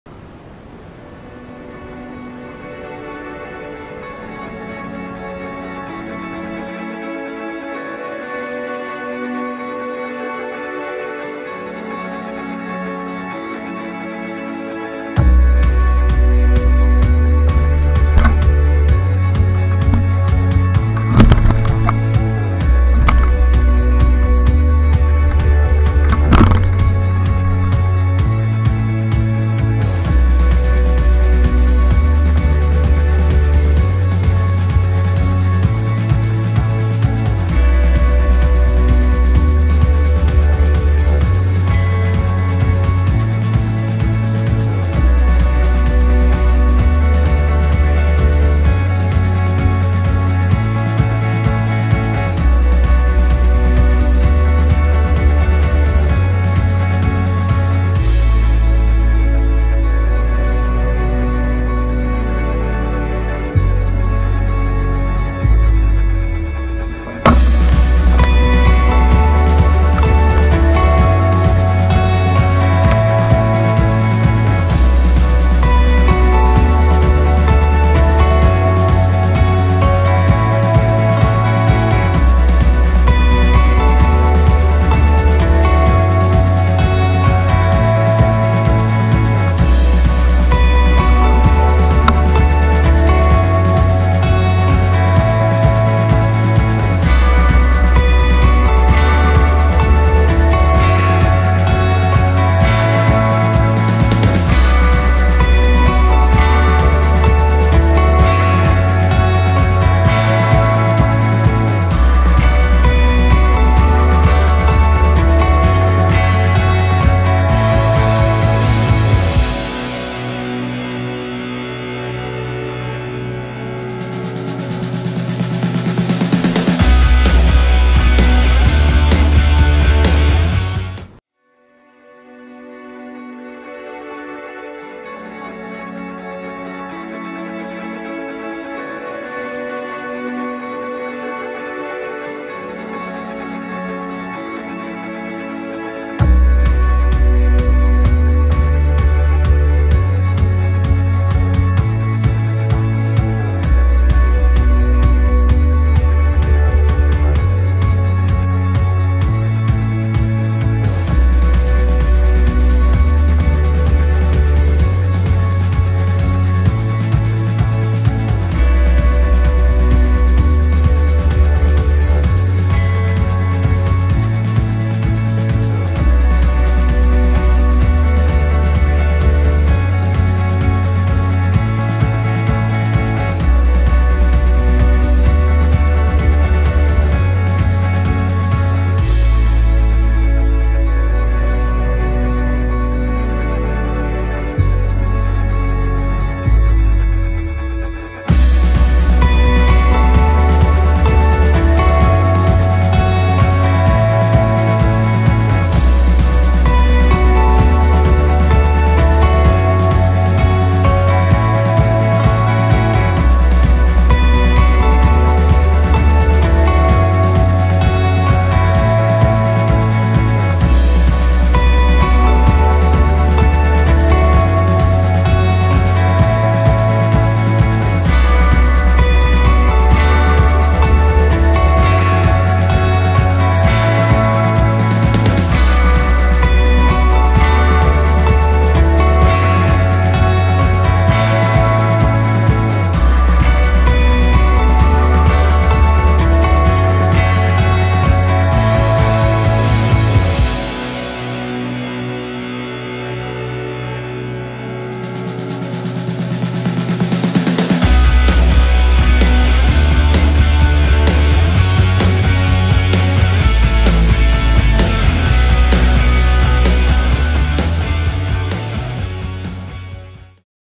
suppen in Spanbroek 2019
suppen bij de nieuwe haven en tevens amperplaats